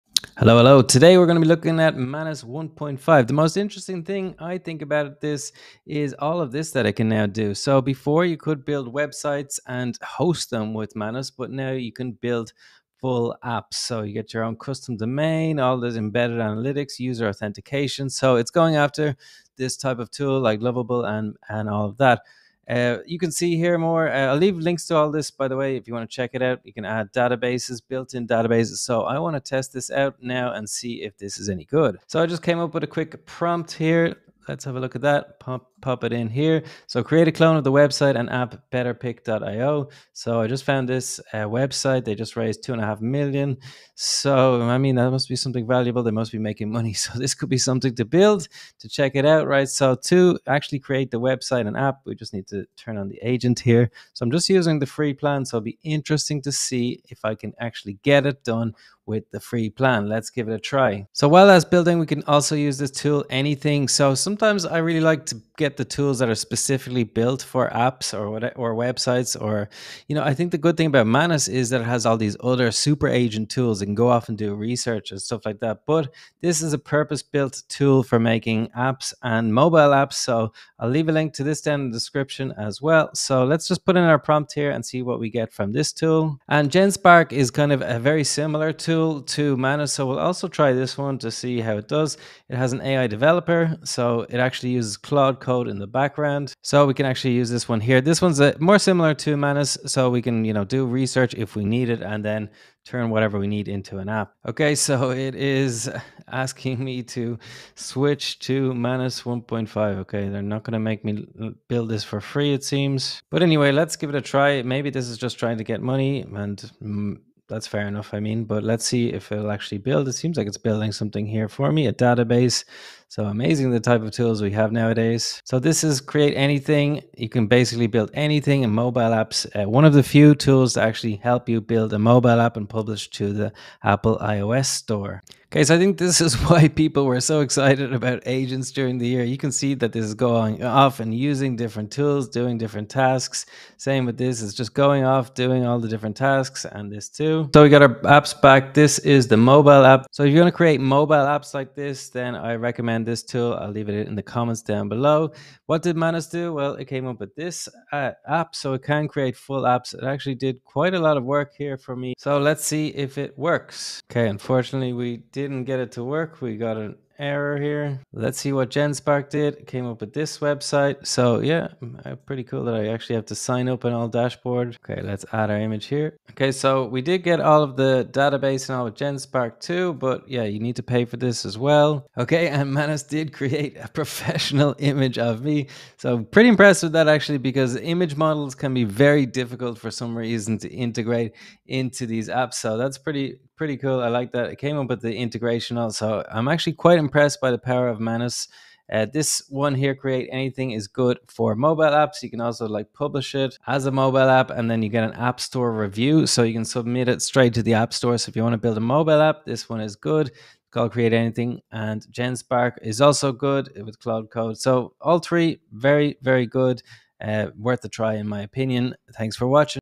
Manus 1.5 Builds Apps Now
Manus 1.5 is a major upgrade to the Manus AI agent platform that delivers significant improvements in speed, reliability, and overall quality across various task types. It runs tasks nearly four times faster than before, reducing the average completion time from about 15 minutes to under 4 minutes. The update includes enhanced intelligence and expanded context handling, allowing the agent to manage larger and more complex projects without losing important details, which benefits team collaboration by maintaining coherence across longer workflows.

A key highlight of Manus 1.5 is its ability to build and deploy fully functional, production-ready web applications from just a natural language prompt. This includes generating complete backend infrastructure, user authentication systems, integrated databases, event-driven notifications, and embedded AI features like multimodal language models and image generation. The platform supports live visual editing where users can point to sections in a live preview and make changes via natural language, custom domain connections for app publishing, built-in analytics for tracking visitor behavior without third-party tools, and robust permission controls to manage access.

Manus 1.5 also offers version control to roll back to any previous app version and enables a seamless, all-in-one workflow for web development that integrates deep research, content creation, app building, deployment, user interaction analysis, and even automated presentations. The platform is designed to empower both individual creators and teams to rapidly create complex, AI-powered web applications without coding or drag-and-drop, simply by describing their ideas in natural language. There’s a full version offering maximum power and a Lite version optimized for cost efficiency and routine tasks. Overall, Manus 1.5 redefines app development by combining speed, precision, and comprehensive AI-driven functionality in a single, continuous-context agent system.